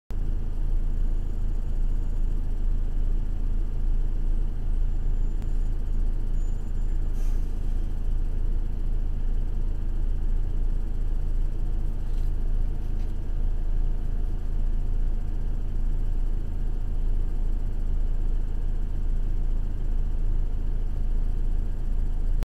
JafaCam: How many cyclists run a single red light (2018?)
Cyclists running red lights, on the corner of Symonds St and K-road, Auckland.

And yes, I am well aware that the date-time function does not work on this camera: I have subsequently bought a new one.